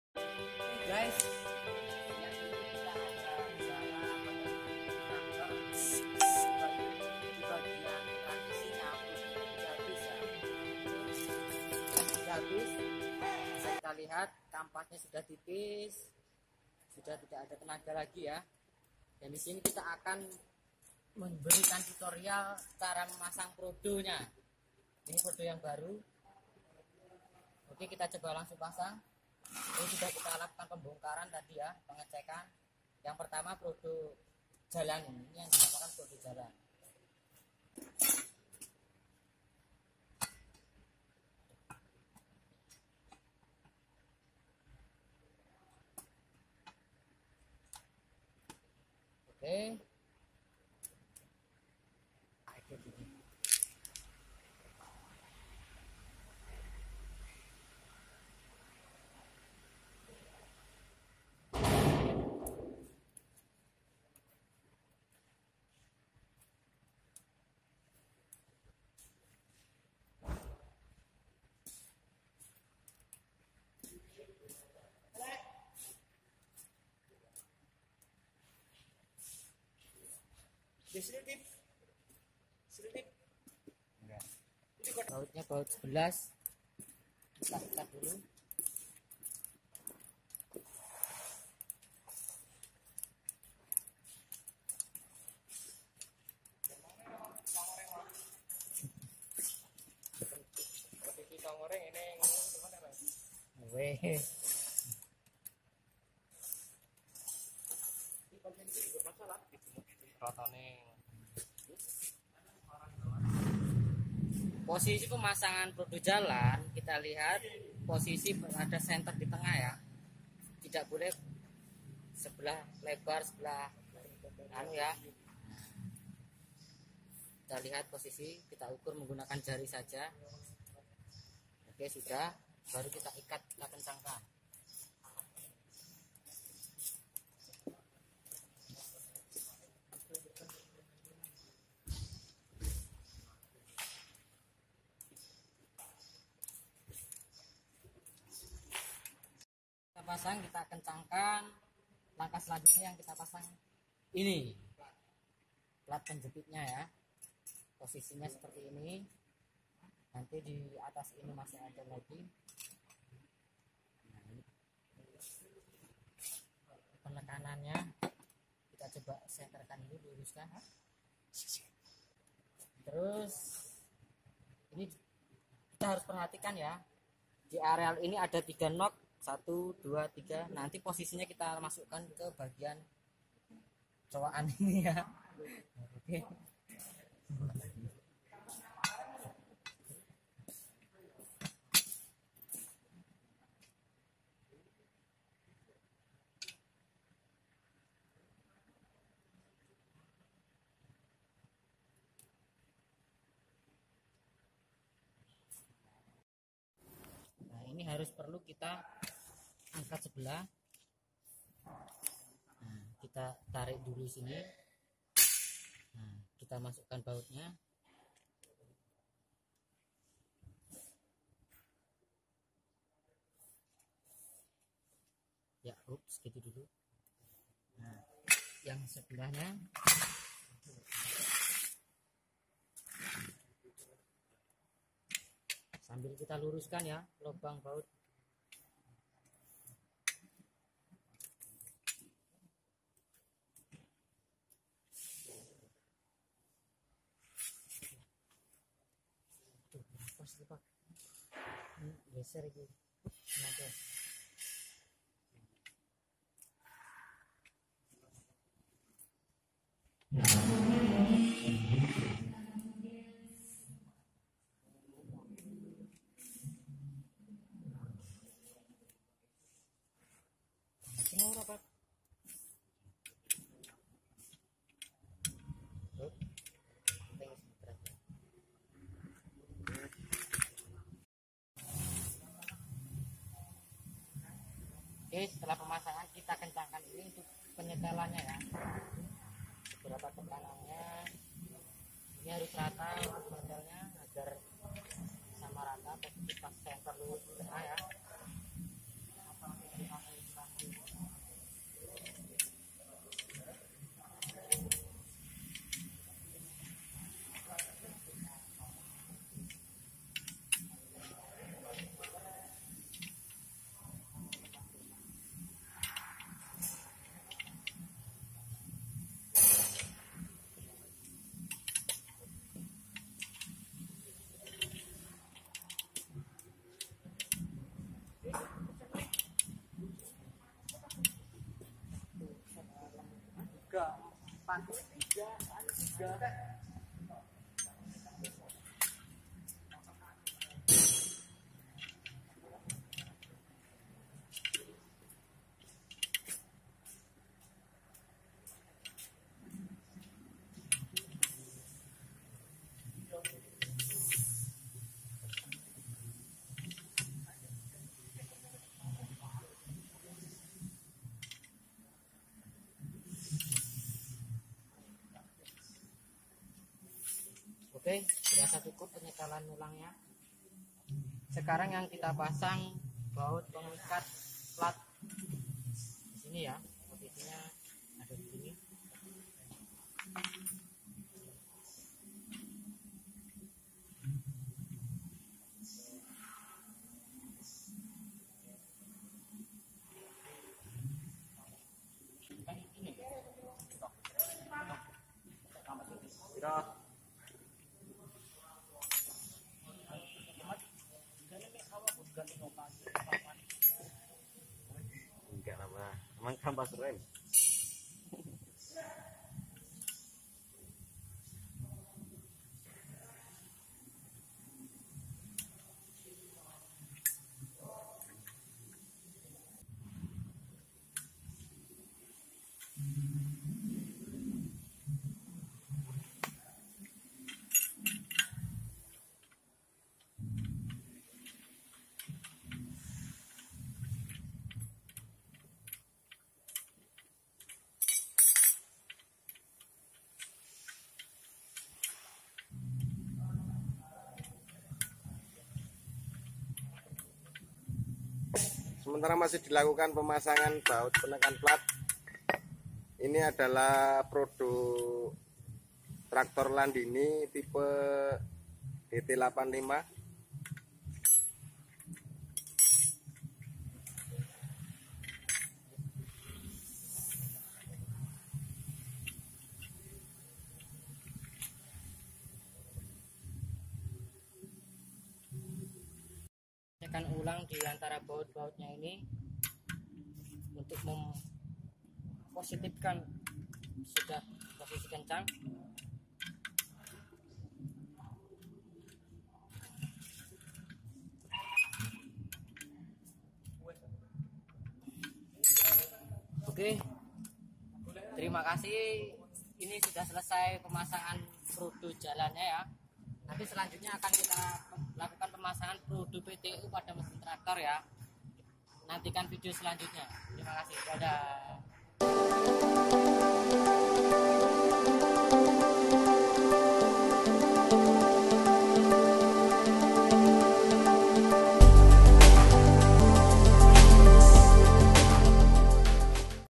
Seputar traktor "PASANG PRODO JALAN TRAKTOR" MANTAAAAAAAAP
proses pemasangan prodo jalam traktor Landini DT85 dari mulai awal hingga akhrir

sistim tranmisi pada traktor landini ini mengunakan tranmisi manual, sehingga harus mengunakan prodo atau sering di sebut juga kampas kopling .
di sini traktor LANDINI DT85 mengunakan 2 kampas kopling yaitu kampas kopling jalan dan kampas kopling PTO yg biasanya di gunakan untuk alat pemotong rumput

maf jika masih banyak kekuranagn